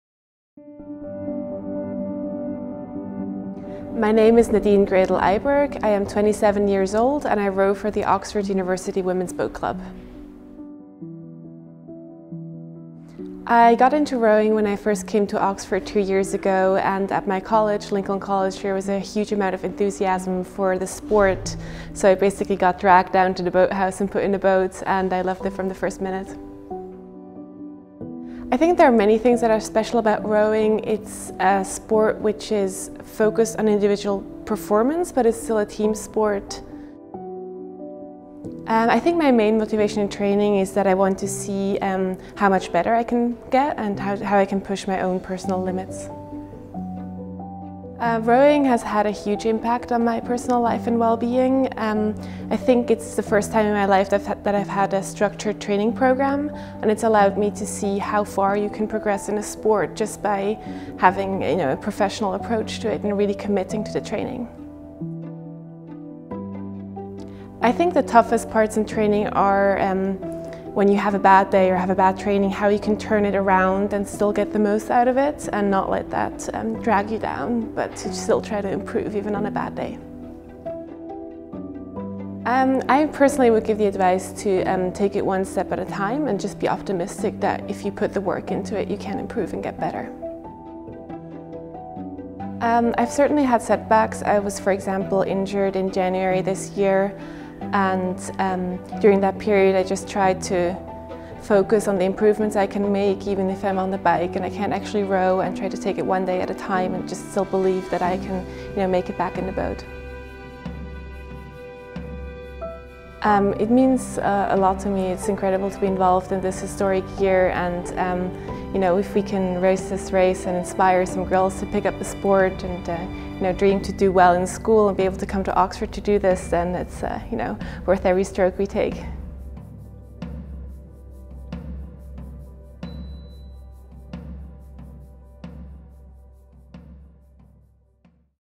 The Boat Race | Nadine Graedel Iberg Profile
Oxford Women’s Blue Boat rower Nadine Graedel Iberg talks to Nuffield Health about her journey to sporting excellence and her desire to inspire other girls to pick up the paddles.

Nadine started rowing just two years ago but her commitment to training and support from team mates has seen her reach the 2015 Oxford and Cambridge Boat Race. She explains her experience in this profile film.